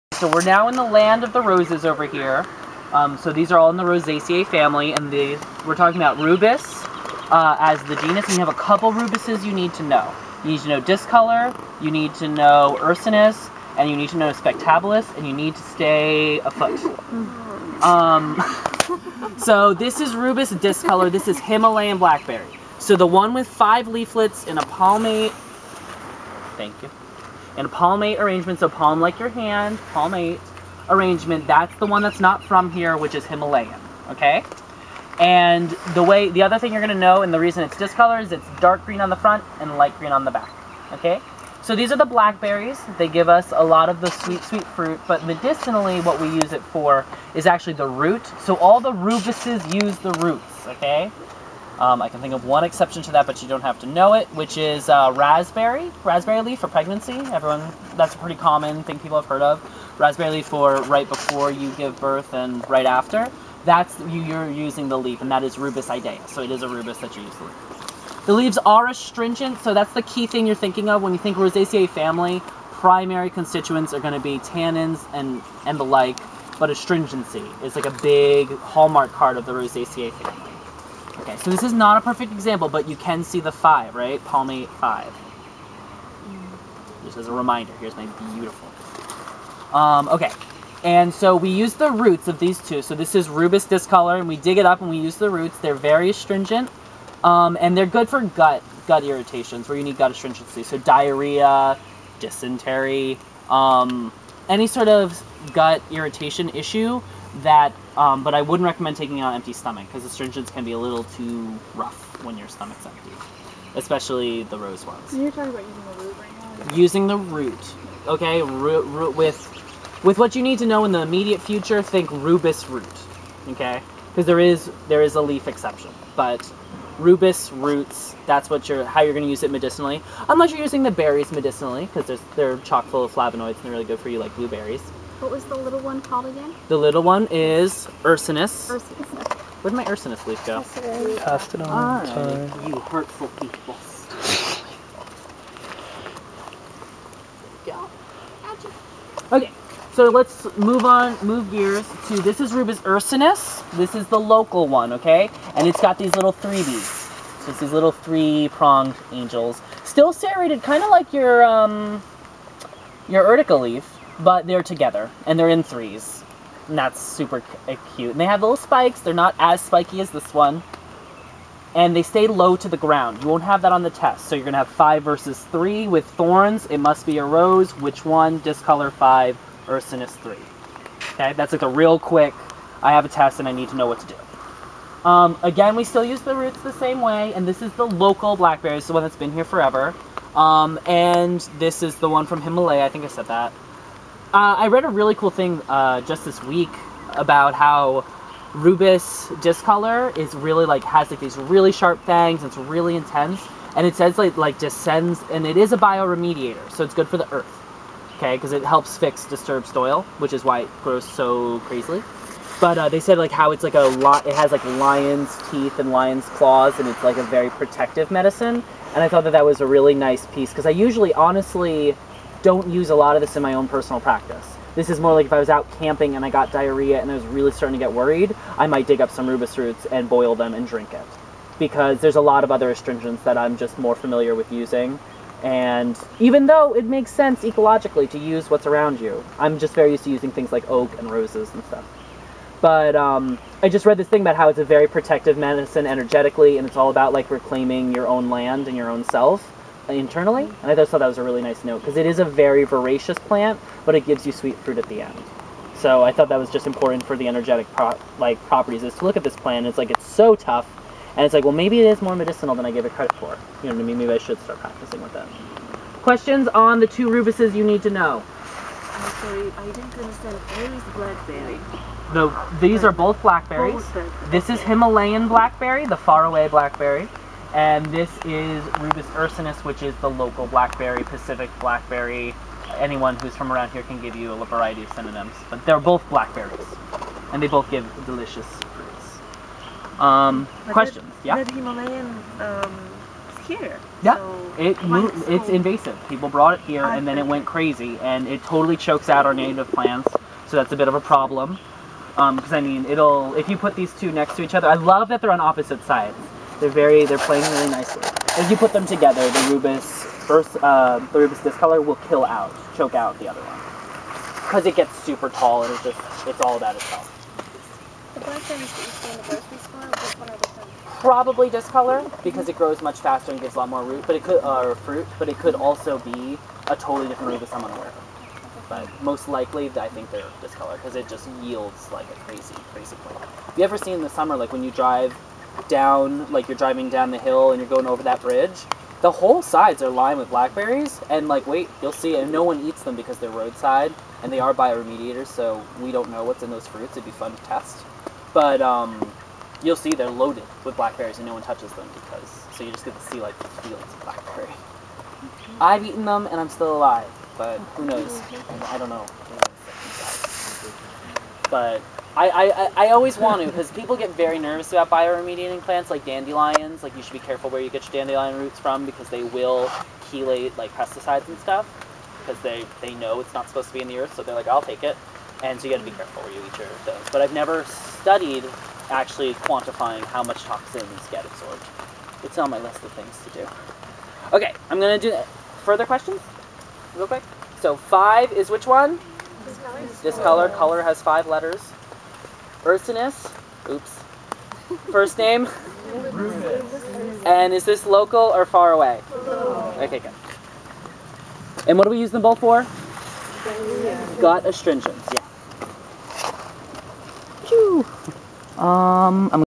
Rubus discolor(Himalayan Blackberry), Rubus ursinus(California Blackberry)
Rubus discolor - Himalayan blackberry - 5 lobed leaf
Rubus discolor- California blackberry - 3 lobed leaf